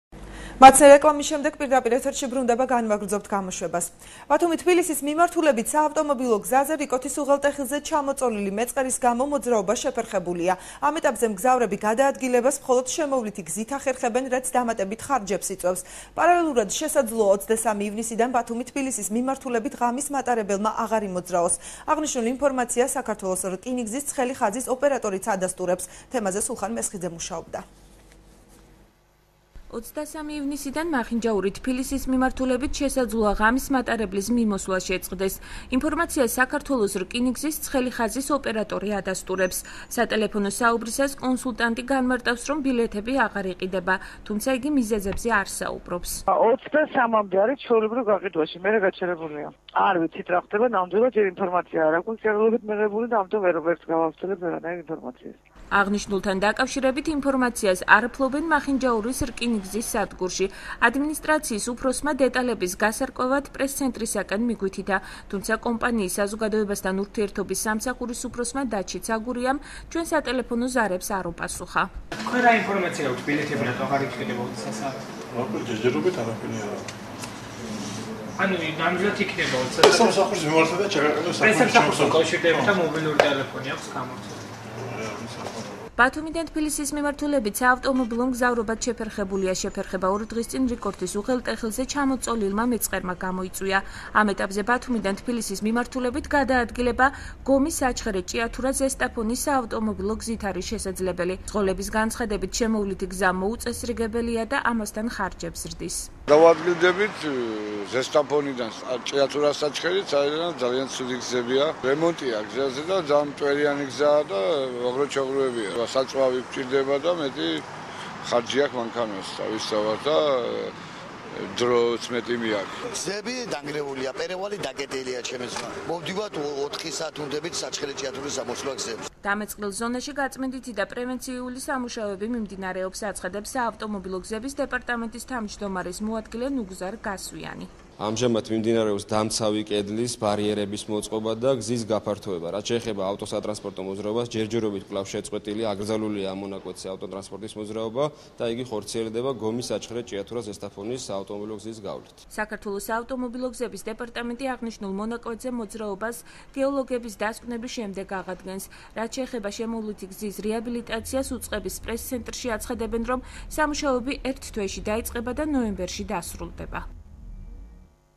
ბათუმი თბილისის მიმართულებით მგზავრობა გართულებულია
ავტორები: სულხან მესხიძე, ლაშა თავდგირიძე, მირზა ციმნარიძე |
23 ივნისიდან მახინჯაური - თბილისის მიმართულებით, შესაძლოა, ღამის მატარებლის მიმოსვლა შეწყდეს. ინფორმაციას საქართველოს რკინიგზის ცხელი ხაზის ოპერატორი ადასტურებს. სატელეფონო საუბრისას კონსულტანტი განმარტავს, რომ ბილეთები აღარ იყიდება, თუმცა იგი მიზეზებზე არ საუბრობს.

აღნიშნულთან დაკავშირებით ინფორმაციას არ ფლობენ მახინჯაურის რკინიგზის სადგურში. ადმინისტრაციის უფროსმა დეტალების გასარკვევად პრესცენტრცენტრისკენ მიგვითითა. თუმცა კომპანიის საზოგადოებასთან ურთიერთობის სამსახურის უფროსმა დაჩი ცაგურიამ ჩვენს სატელეფონო ზარებს არ უპასუხა.

ბათუმიდან თბილისის მიმართულებით საავტომობილო მგზავრობაც შეფერხებულია. შეფერხება ორი დღის წინ რიკოთის უღელტეხილზე ჩამოწოლილმა მეწყერმა გამოიწვია. ამ ეტაპზე ბათუმიდან თბილისის მიმართულებით გადაადგილება გომი-საჩხერე-ჭიათურა-ზესტაფონის საავტომობილო გზით არის შესაძლებელი. მძღოლების განცხადებით, შემოვლითი გზა მოუწესრიგებელია და ამასთან, ხარჯებს ზრდის.

დამეწყრილ ზონაში გაწმენდითი და პრევენციული სამუშაოები მიმდინარეობს- აცხადებს საავტომობილო გზების დეპარტამენტის თავმჯდომარის მოადგილე ნუგზარ გასვიანი.

საქართველოს საავტომობილო გზების დეპარტამენტი აღნიშნულ მონაკვეთზე მოძრაობას გეოლოგების დასკვნის შემდეგ აღადგენს. რაც შეეხება შემოვლითი გზის რეაბილიტაციას, უწყების პრესცენტრში აცხადებენ, რომ სამუშაოები ერთ თვეში დაიწყება და ნოემბერში დასრულდება.